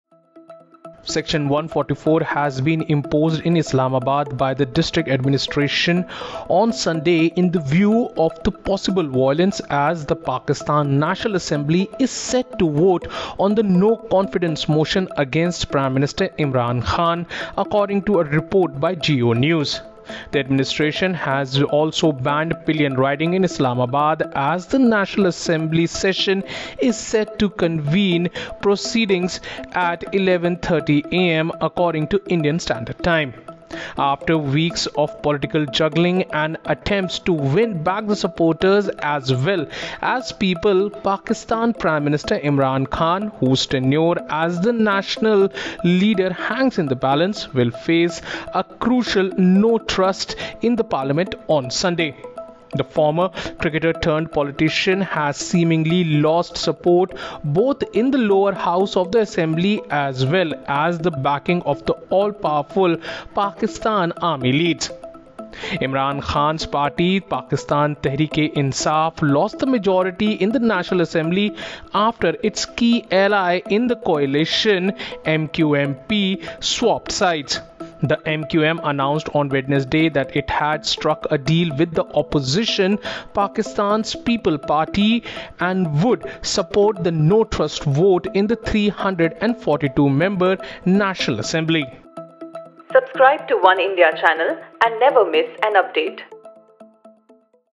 Imran Khan’s No Trust Vote: Section 144 imposed in Islamabad ahead of voting |Oneindia News
Ahead of the no-trust voting against Pakistan’s Prime Minister Imran Khan in the National Assembly, the district administration has imposed section 144 in the area.